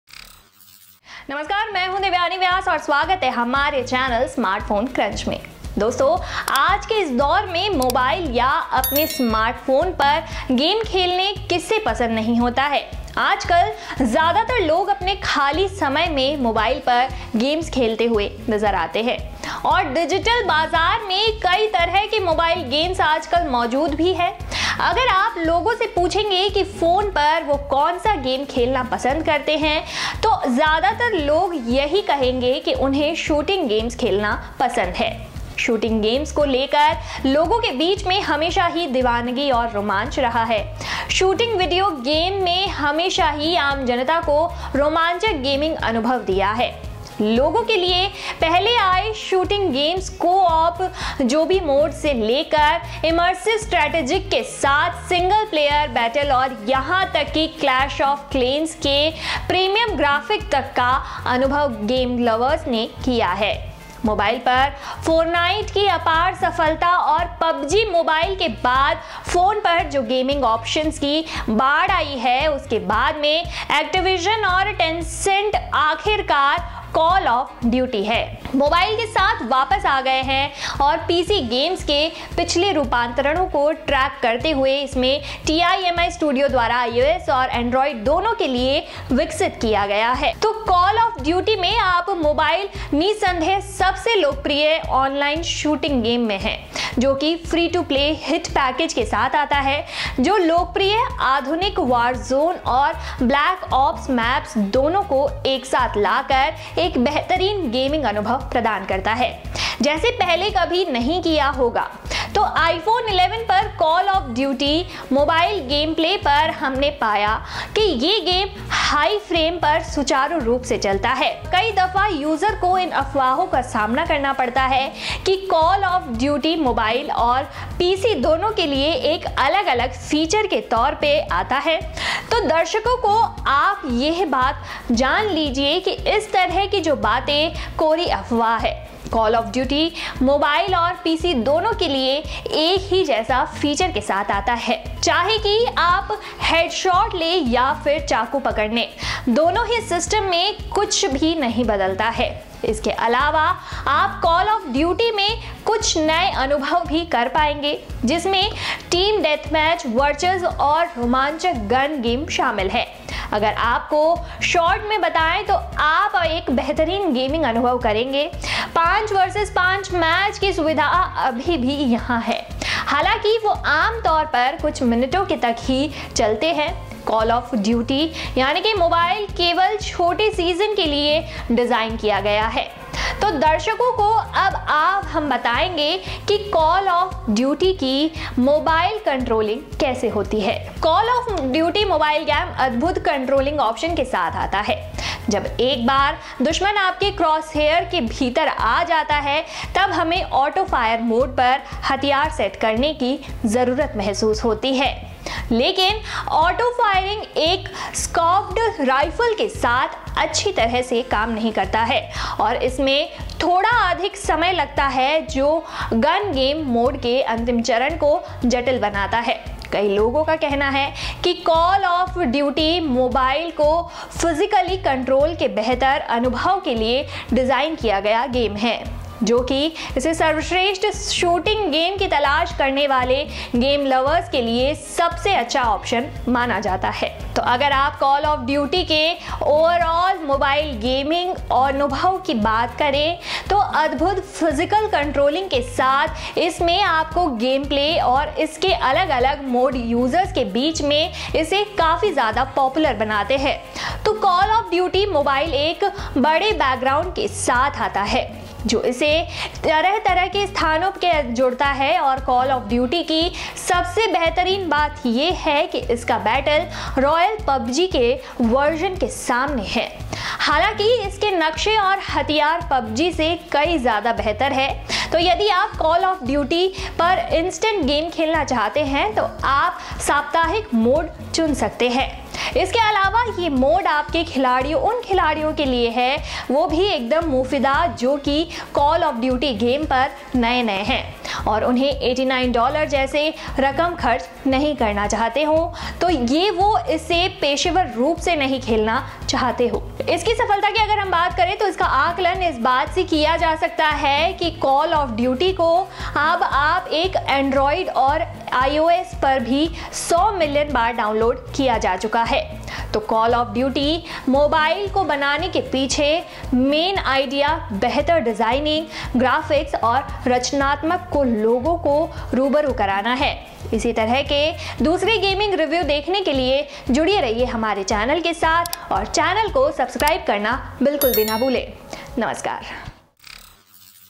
Call Of Duty Mobile Review: The Ultimate Shooting Game
In this video,  you will get a complete Call Of Duty Mobile Review in an exciting manner. The mobile version of this game is available for both Android and iPhone users.

Call of Duty: Mobile is a widely played shooting battle game that has over 100 million downloads and comes with the same flow, features, and graphics as its counterparts on PC and console.

The video talks about the game’s mobile controlling and overall gaming experience. To know more in detail